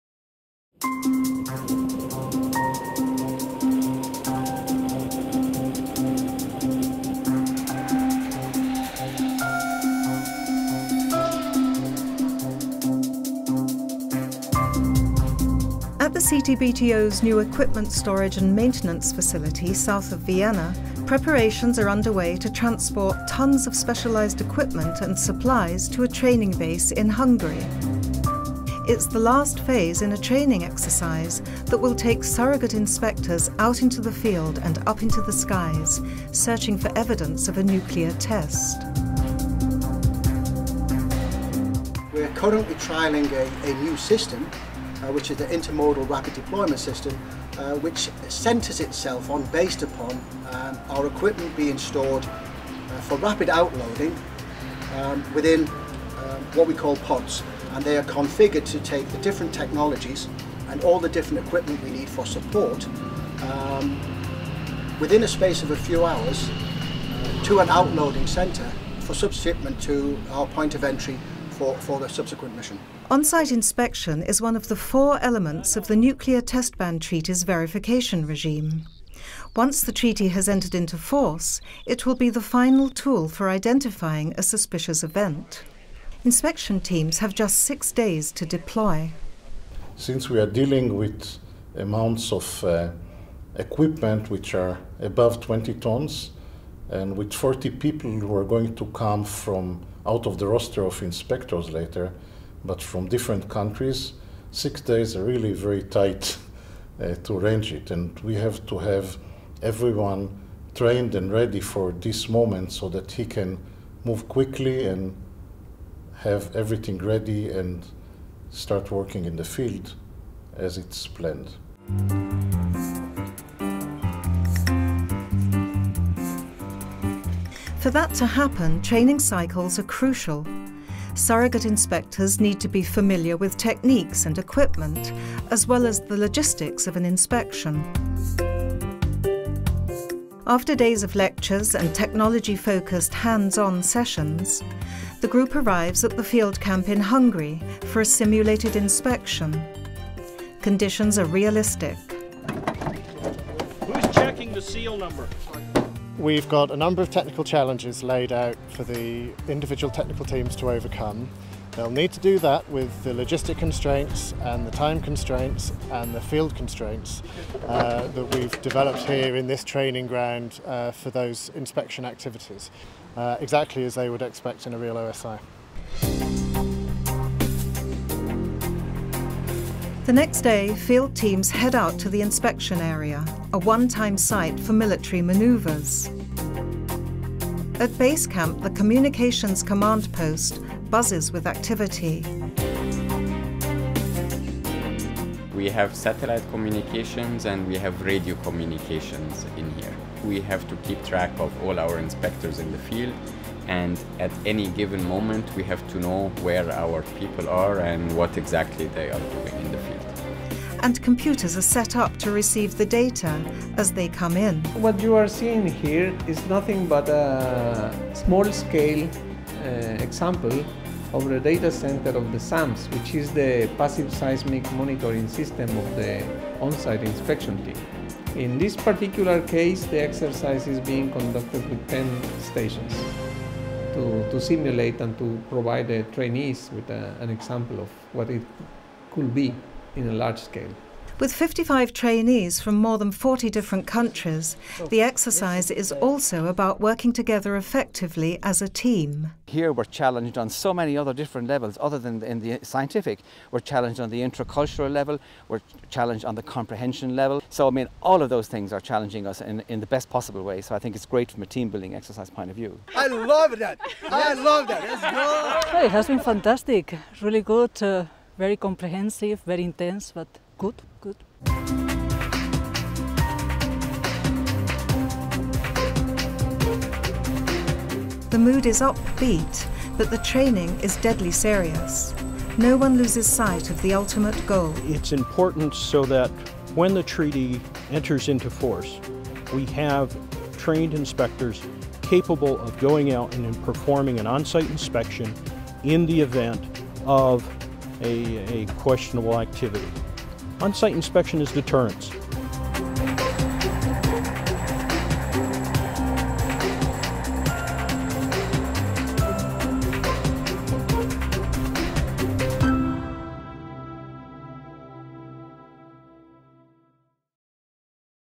Training Inspectors -- Preparing for the Future
Training on-site inspectors for field operations.

Once the CTBT enters into force, on-site inspection (OSI) will be a vital tool in the Treaty's verification mechanism.  To achieve OSI readiness, training inspectors is of paramount importance.  They need to be familiar with using specialized equipment, as well as with the logistical and technical aspects of operating in the field.  As seen here, such training exercises demand effort, energy and team-work.